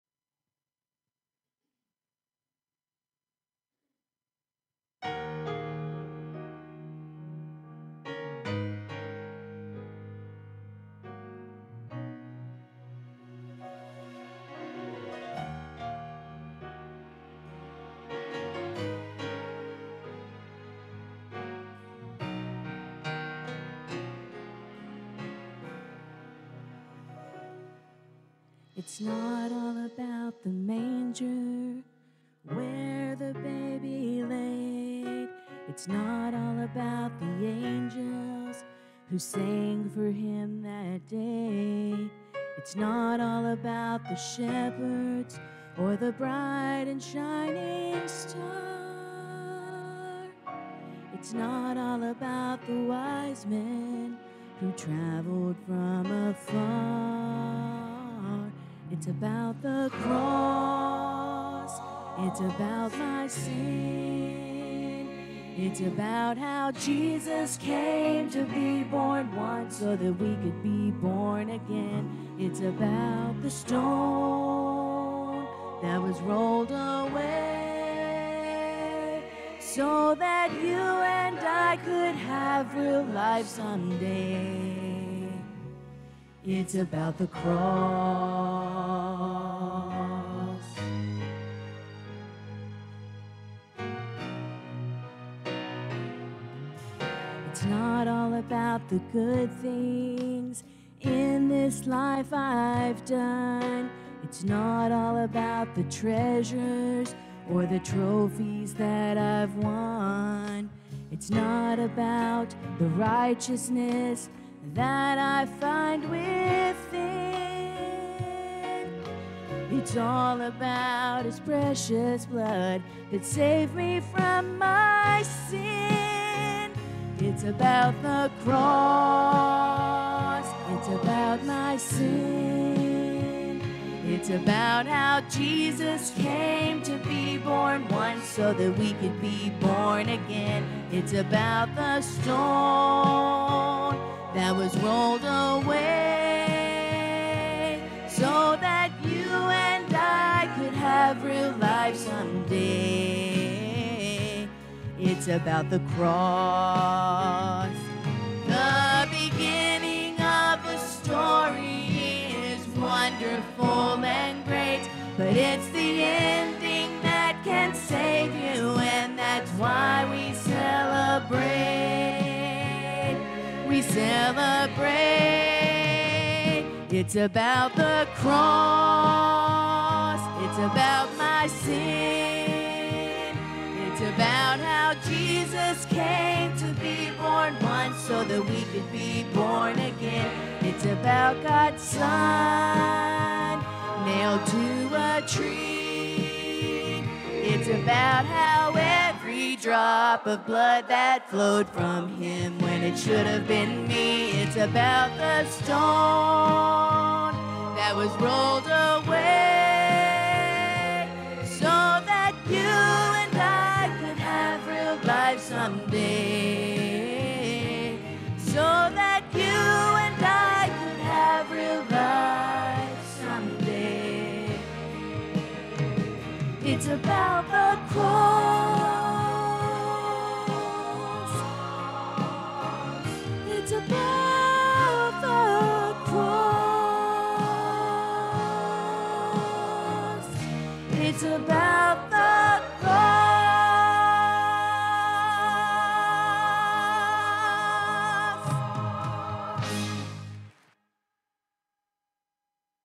Mary Langley's Christmas Eve Special - 2023
Recorded live at Brister Baptist Church in Emerson, AR